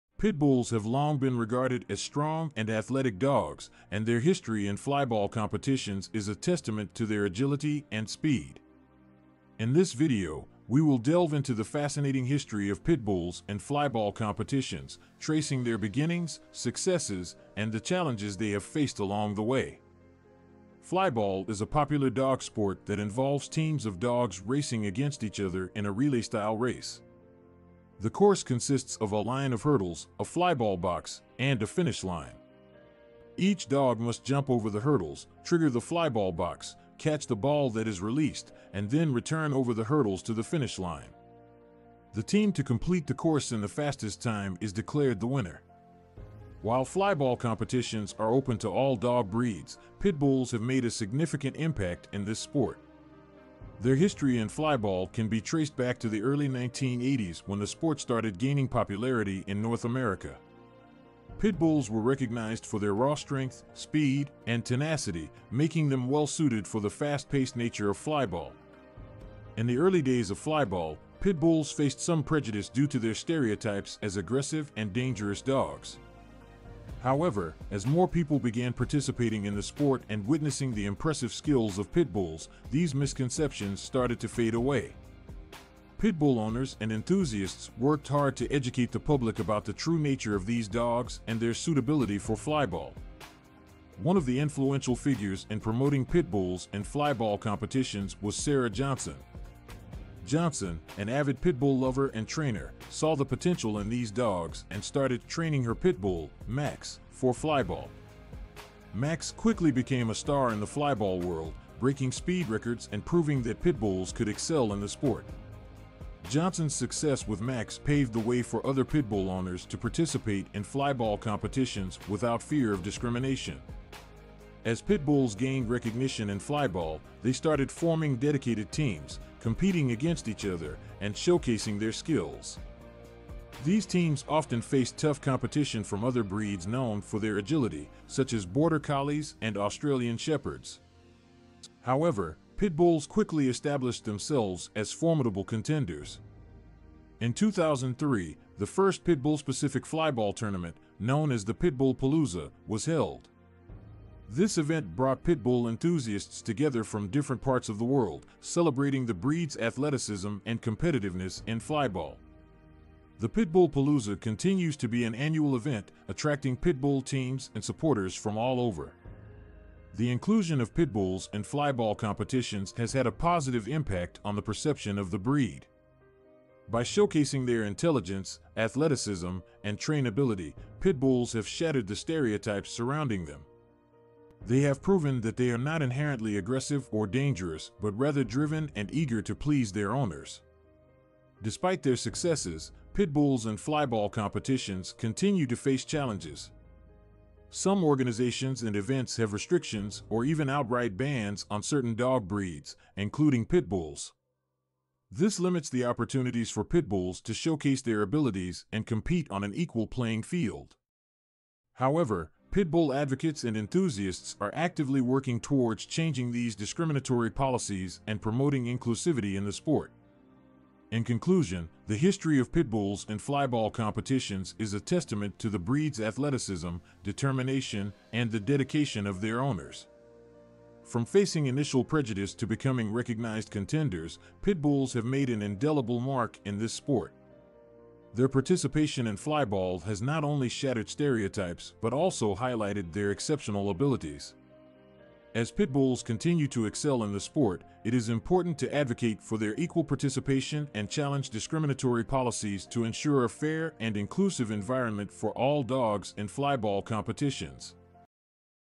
Pitbulls: The Speed Masters of Flyball Racing
Pitbulls have a long history in the fast-paced and thrilling sport of flyball. This video covers the history of these strong and agile dogs in the sport, from their beginnings to their successes and the challenges they have faced.